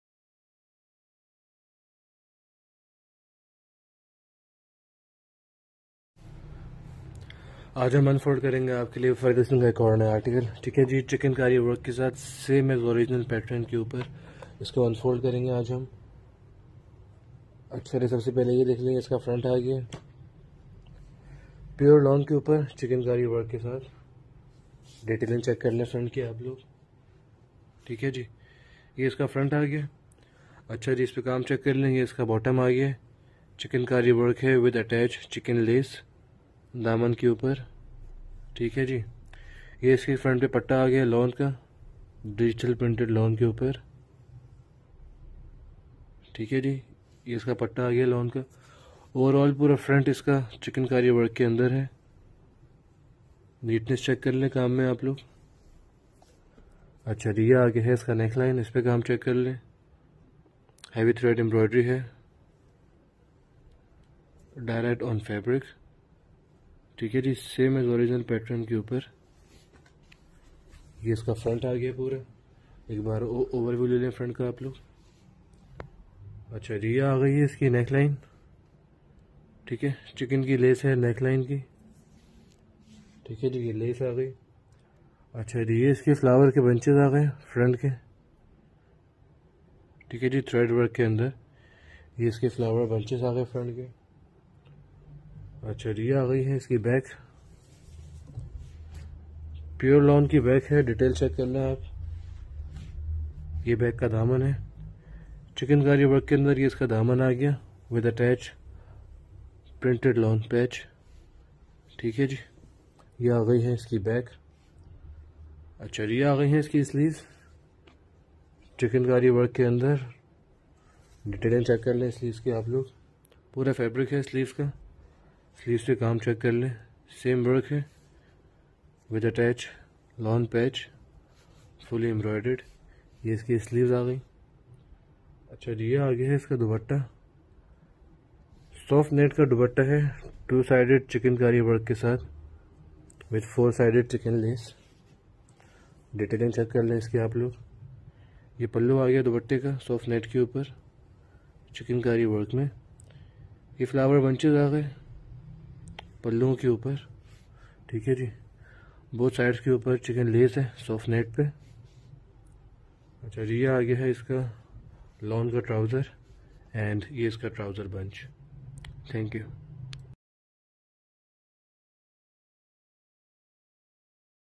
Lawn Suit Design 2021| Summer lawn dress design 2021| Farida Hasan Shiffli Collection
Dress Details :
Brand *(Farida hassan white)*
*JUST IN 4150 FXD*
💣💣 MASTER🔥🔥
Fabric:*FULL Shiffli lawn SUIT*
🔹FRONT EMB SHIFFLI DIGITAL PRINT
🔹BACK DIGITAL PRINT
▶️Dupatta NET SHIFFLI PALLU
▶️SLEEVES EMB ON SHIFFLI
▶️LAWN TROUSER

For More Inquires nd Details